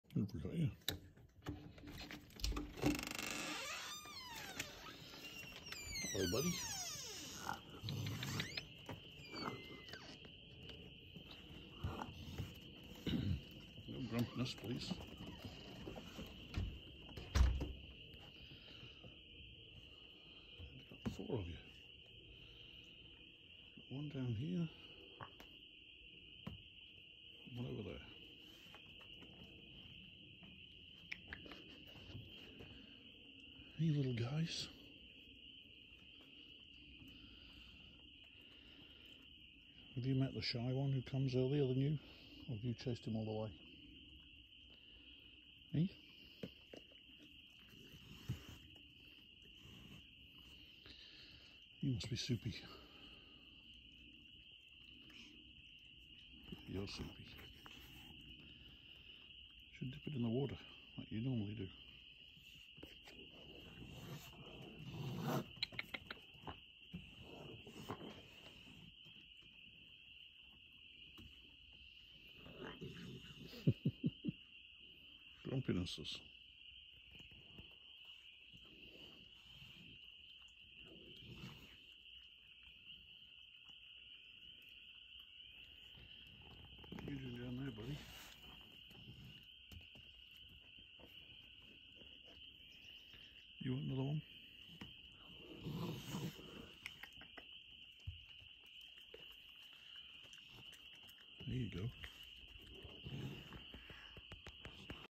Opening the Raccoon Diner for Supper . Peanut Butter Club
The Raccoon Diner opens for the evening . Photography around and about in P.E.I.
Wildlife ,Peanut Butter Club, views and little videos